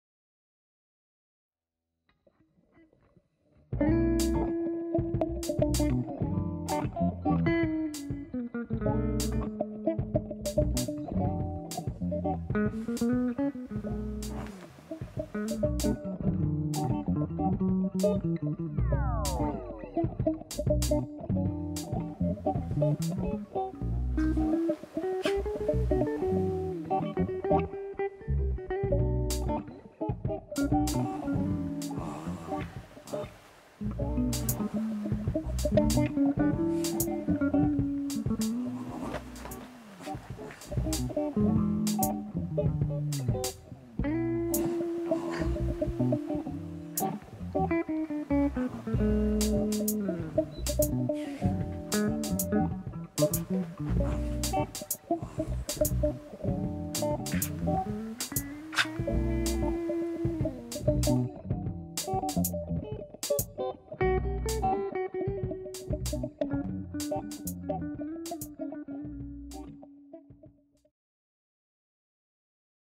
Dog and Toy Moose
recorded this tune and added fluffy dog & toy moose